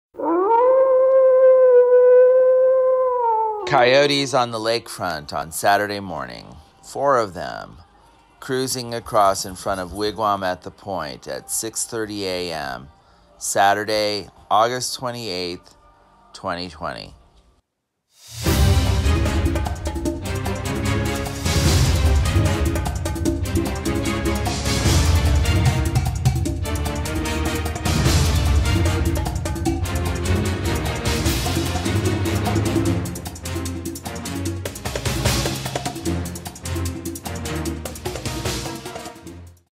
• Coyotes today at 6:30 AM, four of them, cruising across the lakefront. 8/29/2020, Saturday
Good Saturday Morning!
• Coyotes today at 6:30 AM, four of them, cruising across the lakefront.
• They look well fed.
• There are some sort of interesting clouds on the horizon this morning.